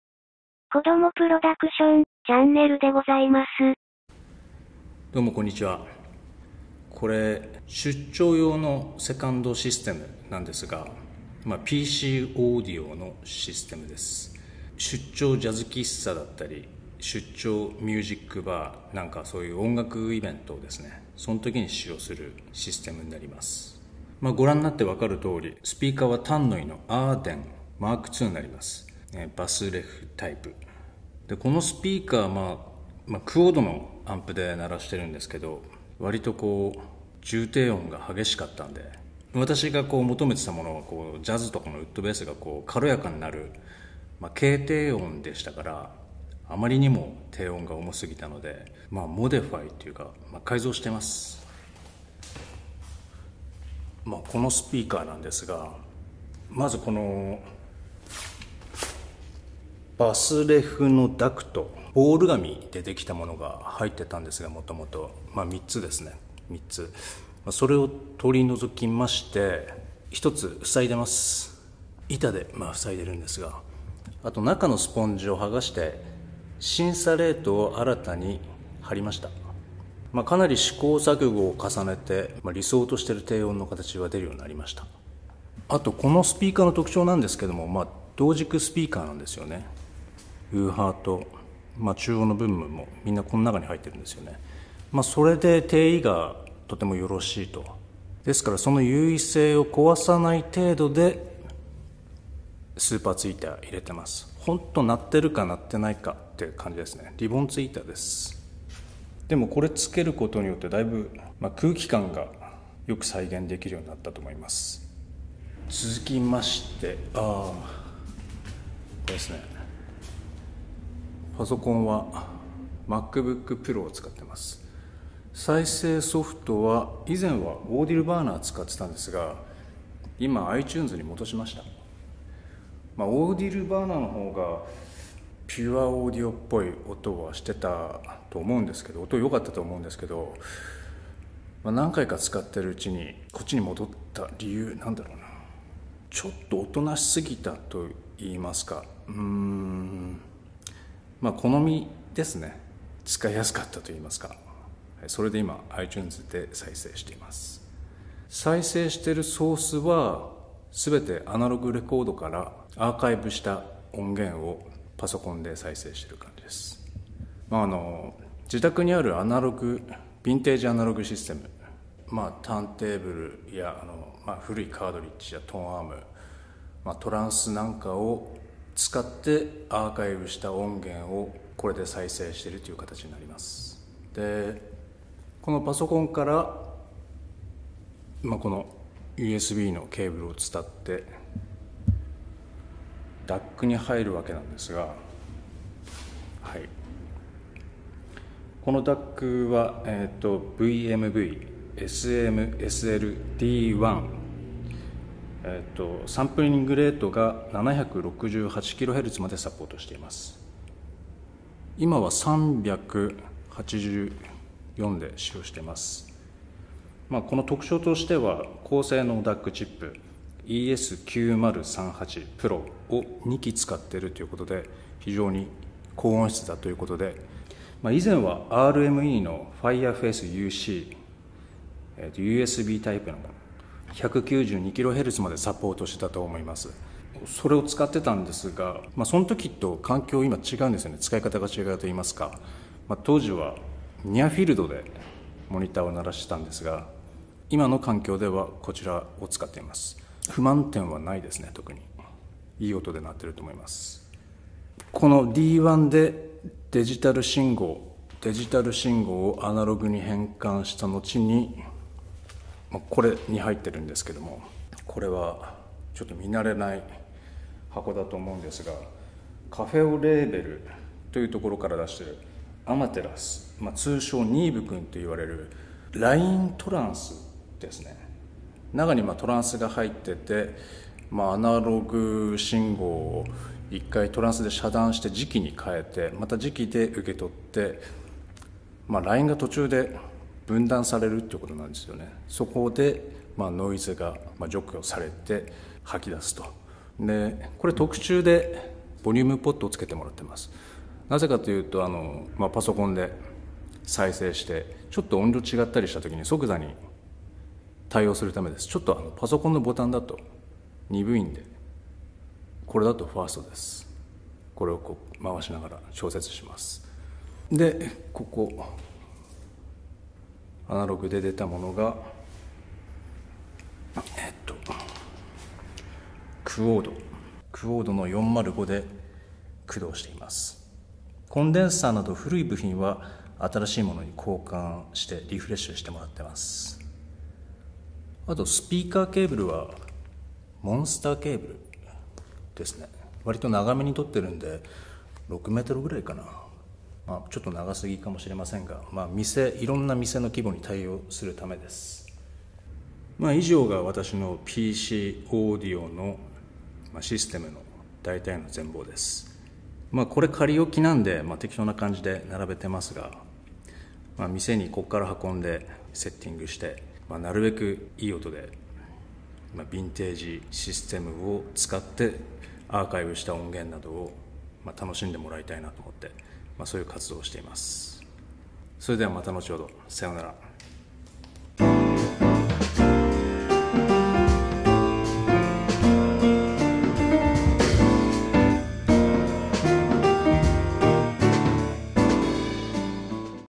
PCオーディオのシステム紹介【オーディオ】PC audio system introduction
以前紹介しましたビンテージオーディオシステムに続き、出張ジャズ喫茶や出張ミュージックバーなどの音楽イベントに使用しているPCオーディオシステムの紹介動画です。
スピーカー１本が４２Kgこれが一人でギリギリ持ち運べるサイズとなります。

ーーー出張用セカンドシステム概要ーーー
TANNOY　ARDEN2（改）バスレフダクト排除、内部吸音材シンサレートに交換
音工房Z スーパーツィーターキット（ペア）
QUAD 405
VMV SMSL D1 DAC
AMATERAS  ニーヴくん(LINEトランス＋ボリューム付）
MacBook Pro（アナログ盤アーカイブデータ使用）

■TANNOY　ARDEN mark２
そのままの状態ではジャズを聴くには少々重い低音だと感じましたので、ダクトの排除を行い内部の古いスポンジ吸音材を剥がしハイテク素材のシンサレートに交換する事によって軽低音（ウッドベースのアタック）がより聞き取りやすくなりローエンドのバランスもイメージに近づける事ができました。
低音の改善が済みましたので定位感を損なわない程度のスーパーツイーターをプラスする事で産毛が見えるようになりました。

今まで使用していたRME Fireface UCに不満もなかったのですが、試しに購入してみたVMV SMSL D1 DACの方がオーディオ的にはシルキーで好みでした。
レコード盤を48khz24bitでアーカイブした音源を再生していますが、レコードを演奏しているのと遜色なく演奏出来ていると感じています。

■AMATERAS  ニーヴくん
音の隙間がより明確になり騒がしくないといいますか良い意味で音が落ち着きます。
メーカーに相談してボリュームを付けていただきました。

作法其の74　子吉川 サクラ鱒 タックル紹介＆誰でも出来る簡単ルアー塗装

🎩✨いつもchannelのsupport有難うございますThank you✨🌹